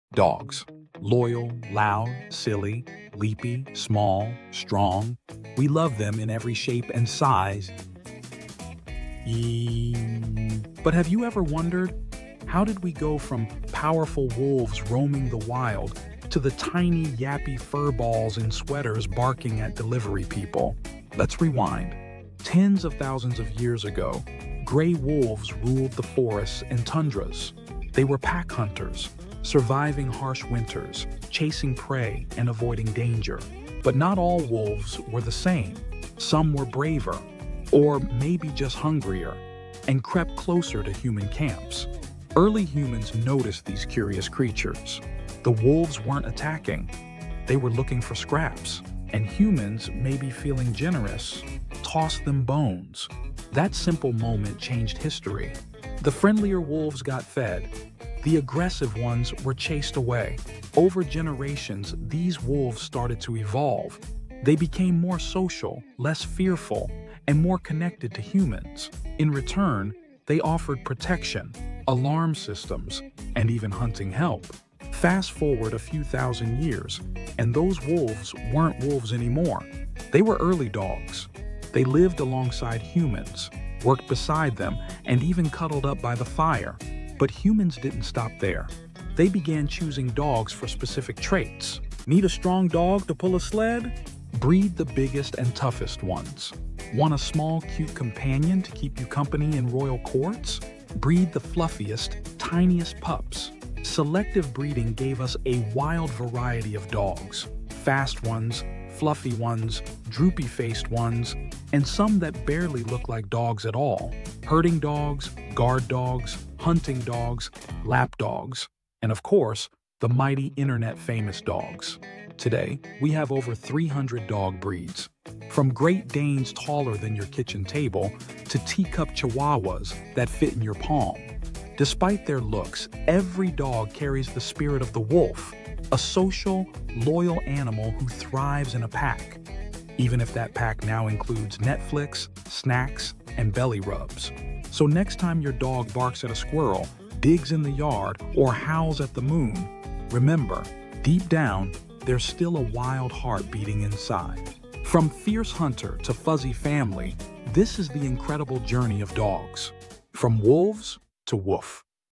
From Wolves to Woof The Evolution of Dogs
🐺➡️🐶 From fierce wolves to tiny yappy pups — discover how dogs became our best friends! This 3-minute journey shows the wild transformation from ancient hunters to modern couch potatoes. 💥

Whether you're a dog lover or just curious about animal evolution, this one's for you.
🎥 Watch now and meet the ancestors behind your furry friend!